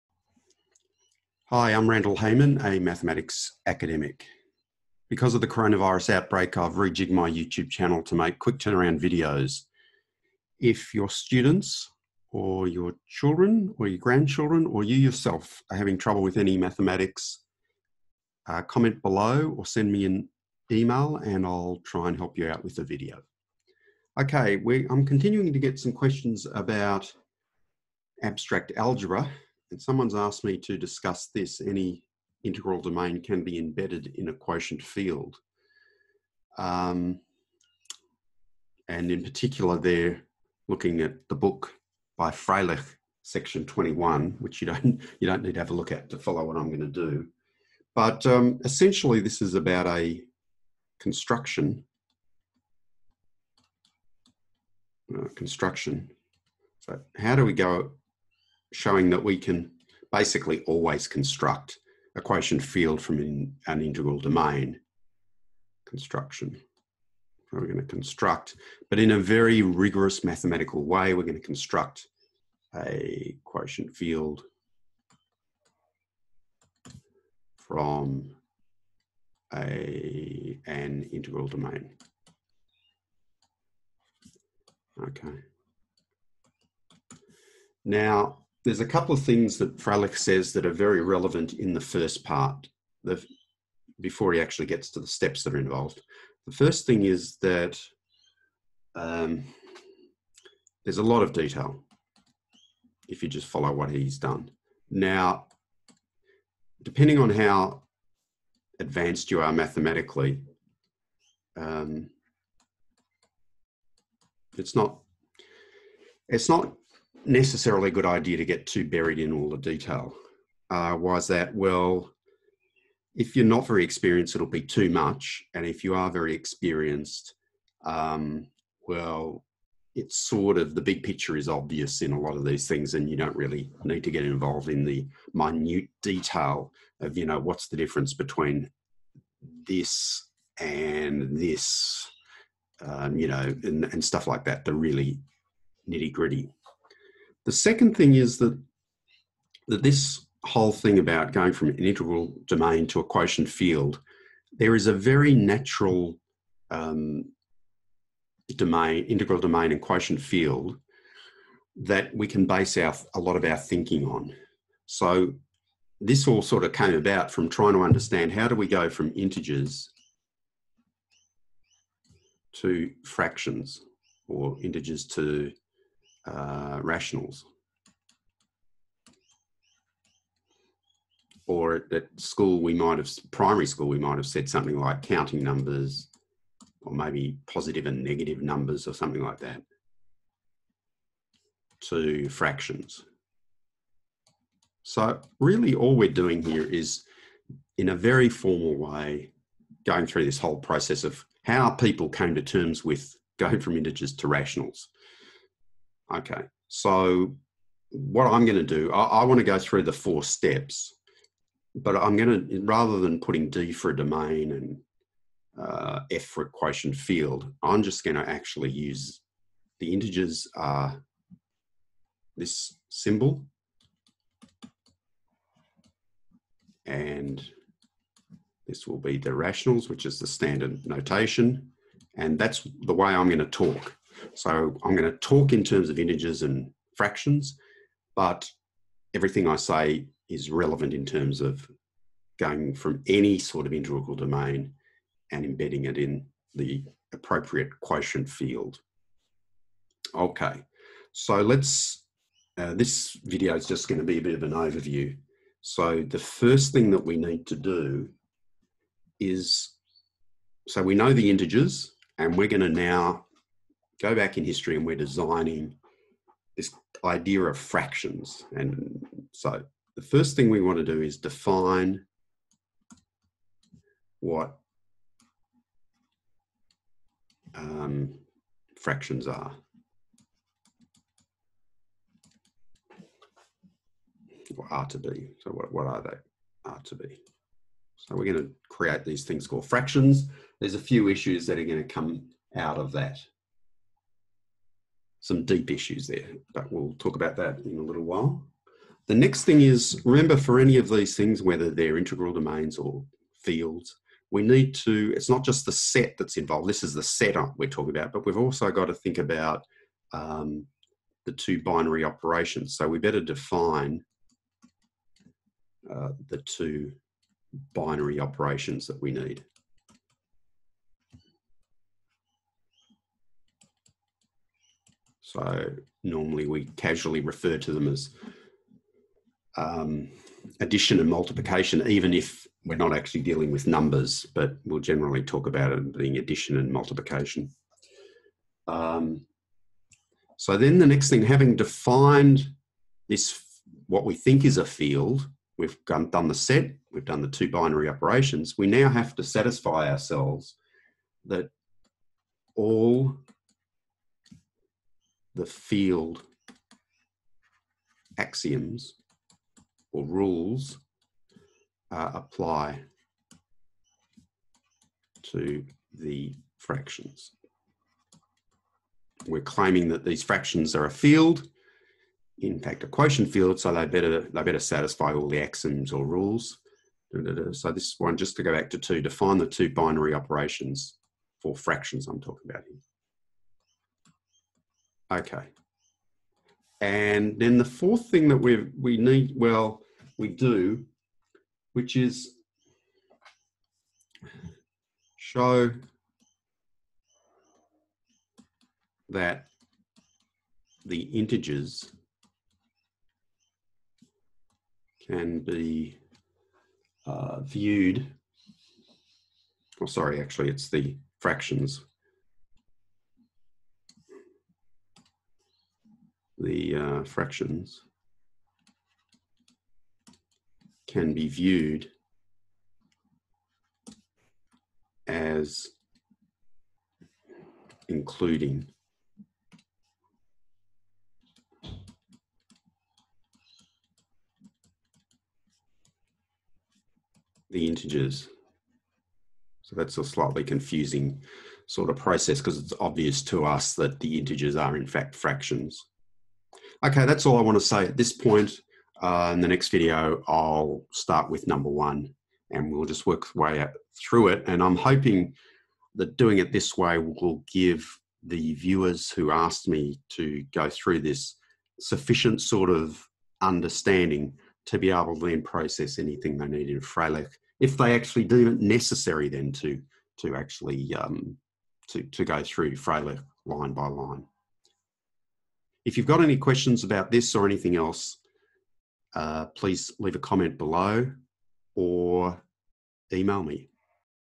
The quotient field of an integral domain - part 1
Responding to a viewer I discuss the four steps to show that every integral domain has a corresponding field, the quotient field. Happy to answer high school and primary school mathematics questions as well!